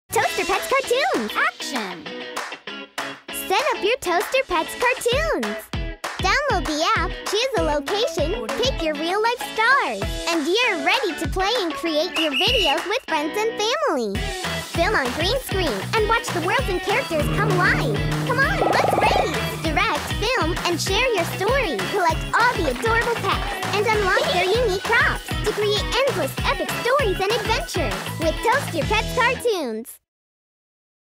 Toaster Pets Cartoons (Animation Studio) - TV Advertisement
Create your High Quality Cartoon with Friends and Family in minutes with Toaster Pets Cartoon Studio. Upload to your Youtube channel awesome cartoons and start you own cartoon network Now!